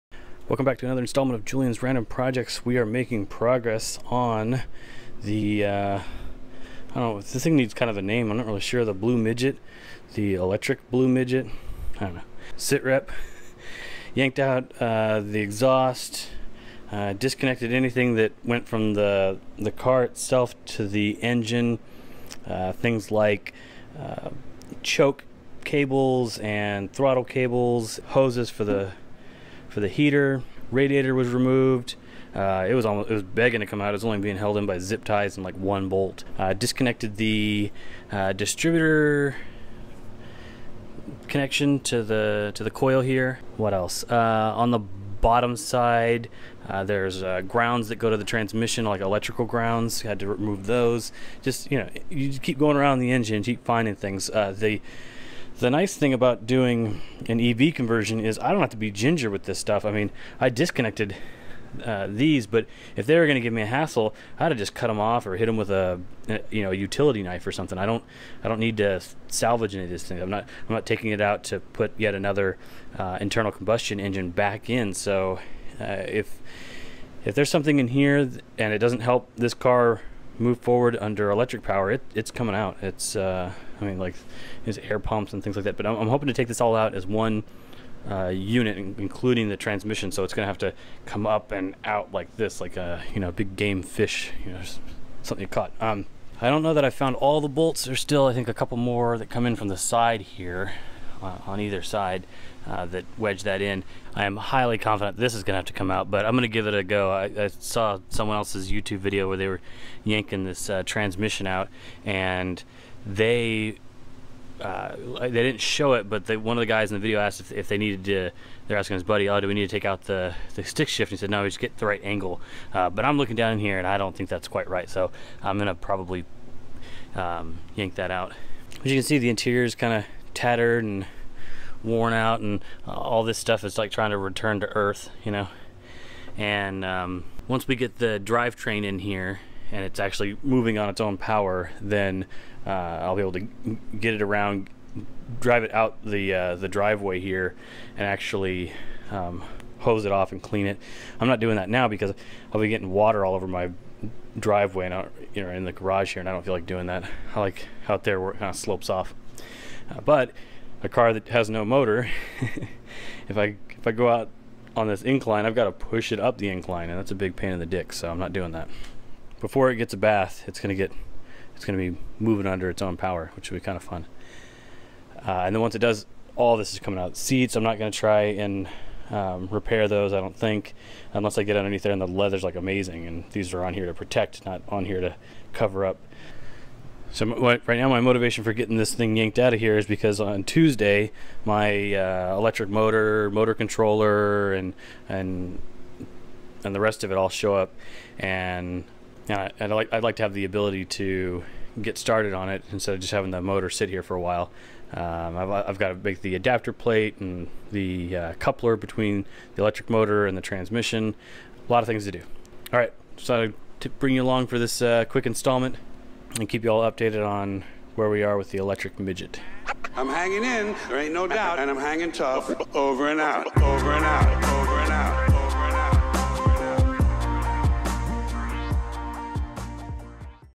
MG Midget EV Conversion Part2
Quick Situation Report on the EV conversion MG Midget. Engine about to come out.